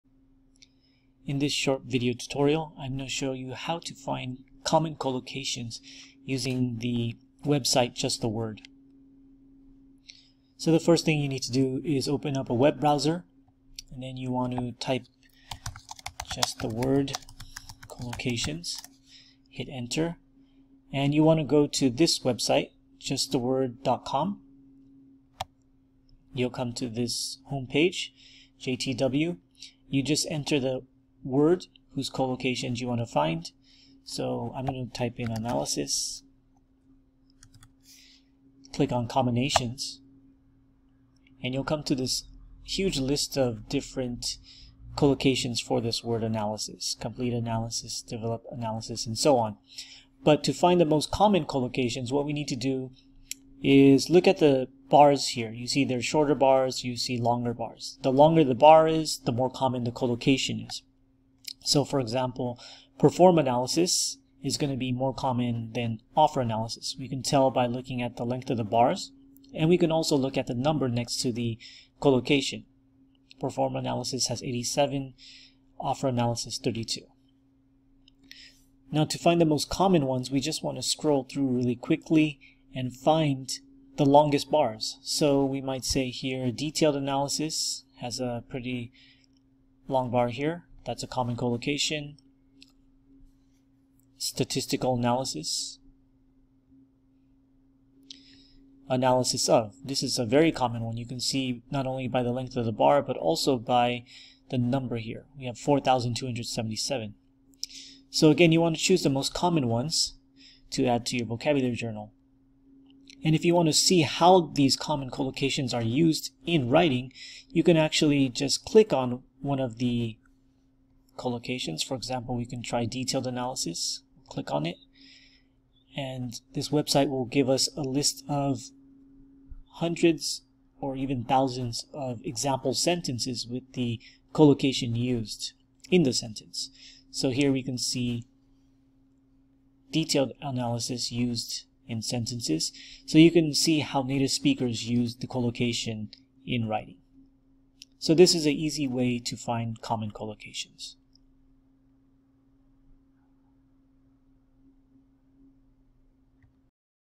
Finding Common Collocations Using 'Just-the-Word' Collocation Finder
This tutorial will show you how to find common collocations using justtheword.com